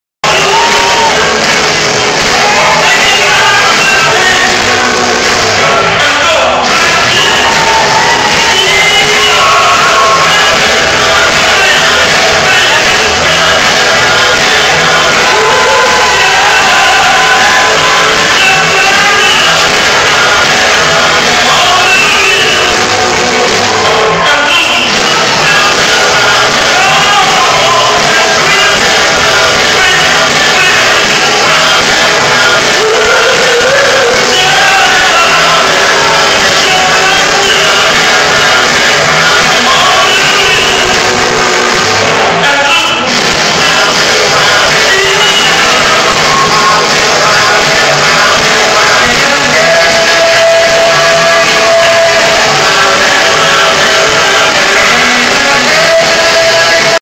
Hula Hooping at Hard Rock Park Myrtle BEach before the Show
KEri having a blast in front of the "crowd" at Hard Rock Park